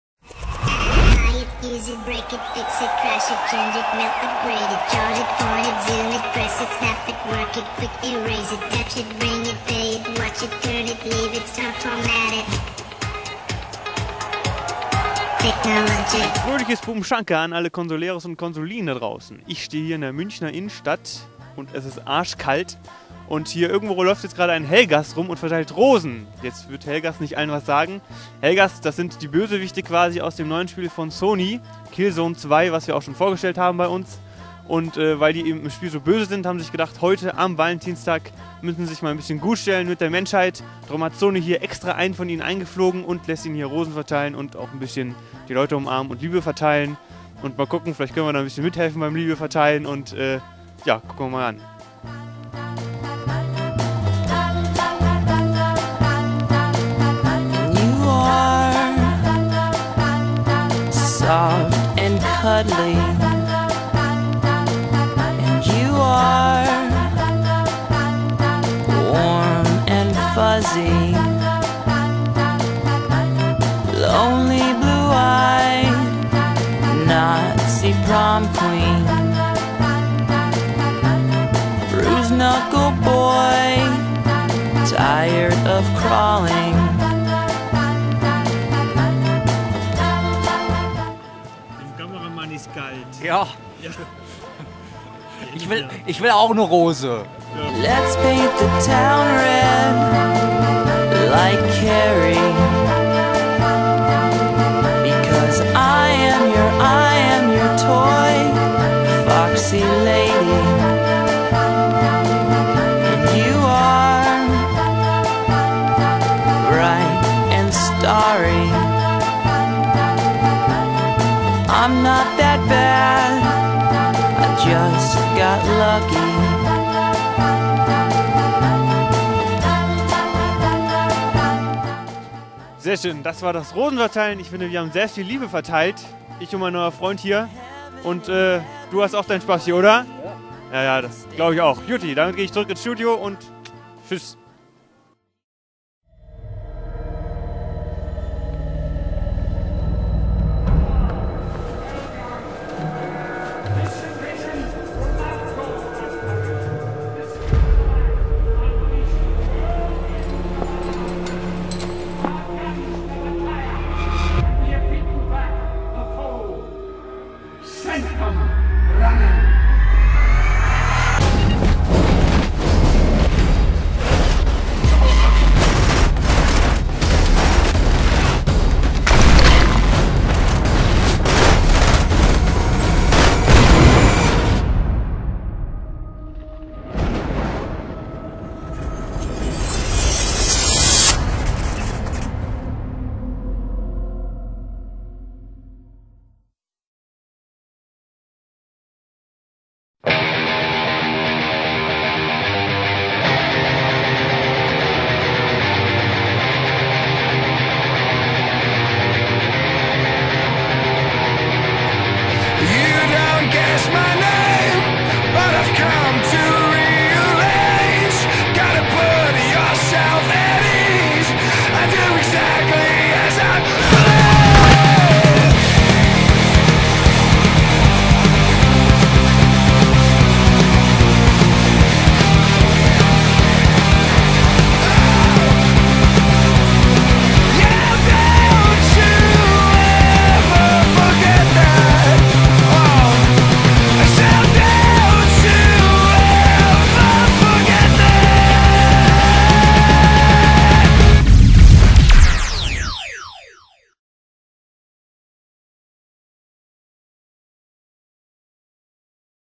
Killzone 2 Valentinsday PR-Action
Zum Valentinstag verschlug es einen einsamen Helghast nach München auf die verschneite Fußgängerzone. Wir konnten natürlich nicht anders als die Kamera zu schnappen und zu drehen, was dieser Krieger denn an greueltaten vor hatte.

Kleiner Hinweis: In diesem Video kann man die Freundin von Hendrik entdecken!